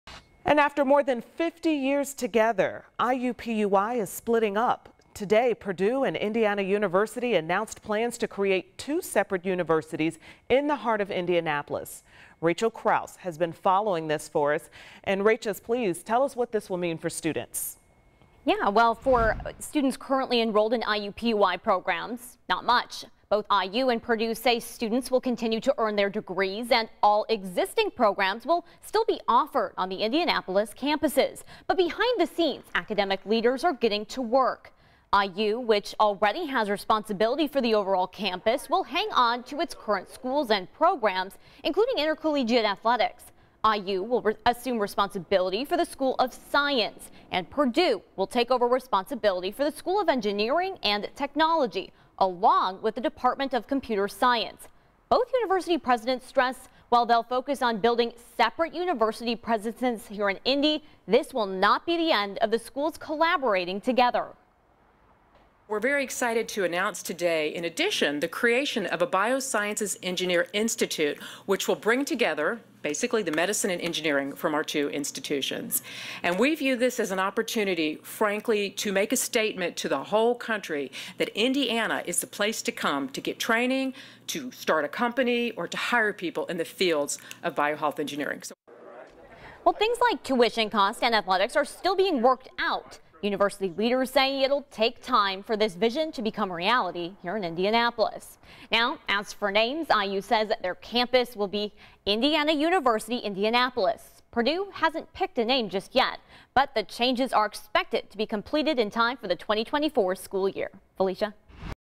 IUPUI splitting into 2 universities
The changes are expected to be completed in time for the fall 2024 semester.